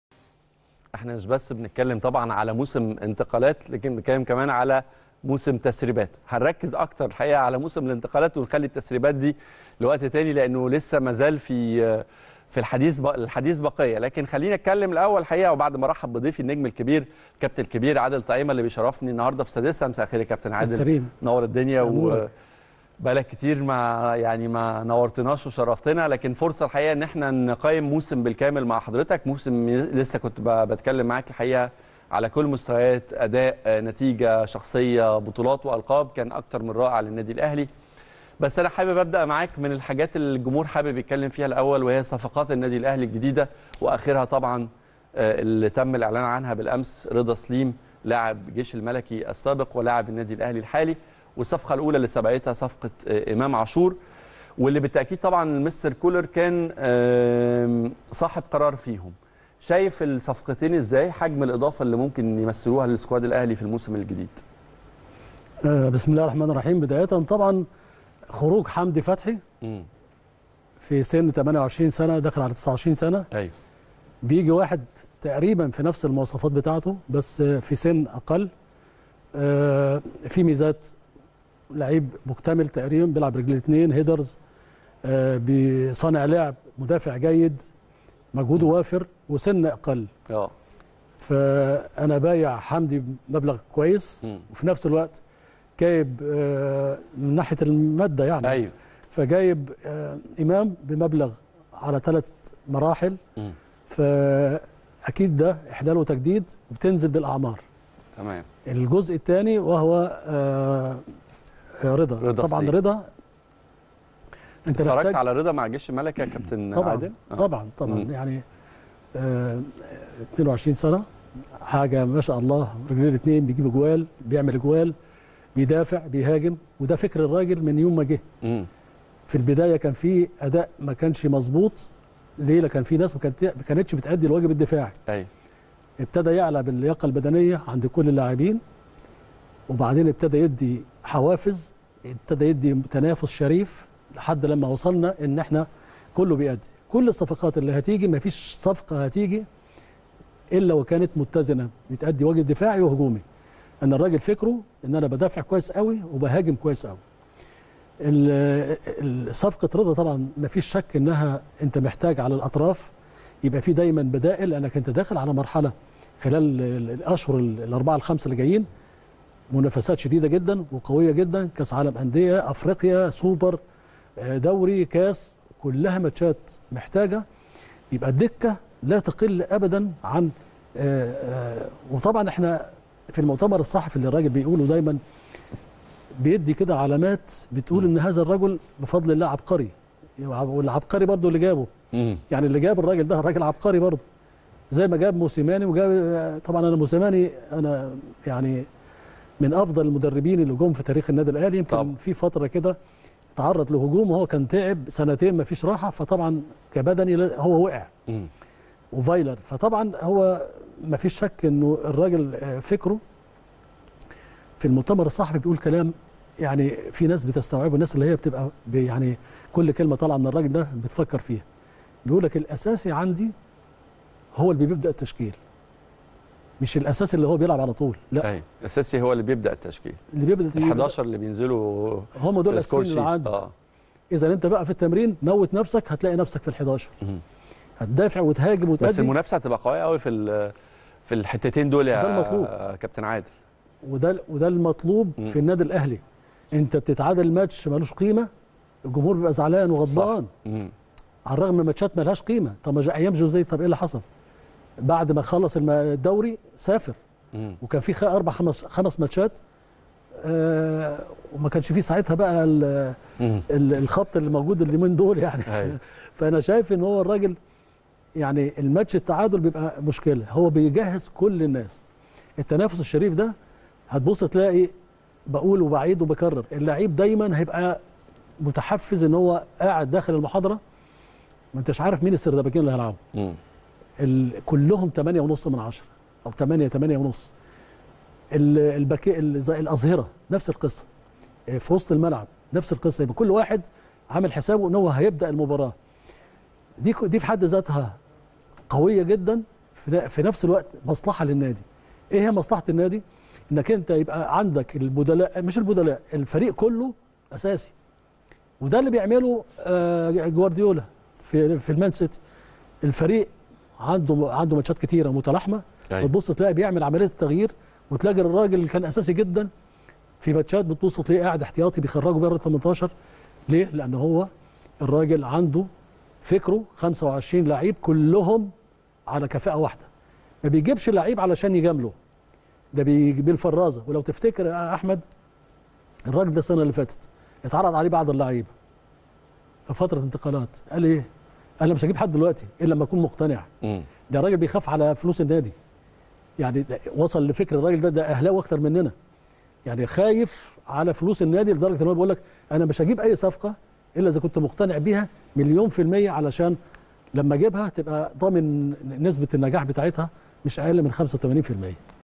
عادل طعيمة وحديث عن صفقات الأهلي الجديدة "إمام عاشور ورضا سليم" وحجم الاضافة التي سيمثلها الثنائي
الكابتن عادل طعيمة وحديث عن صفقات الأهلي الجديدة "إمام عاشور ورضا سليم" وحجم الاضافة التي سيمثلها الثنائي في سكواد الأحمر.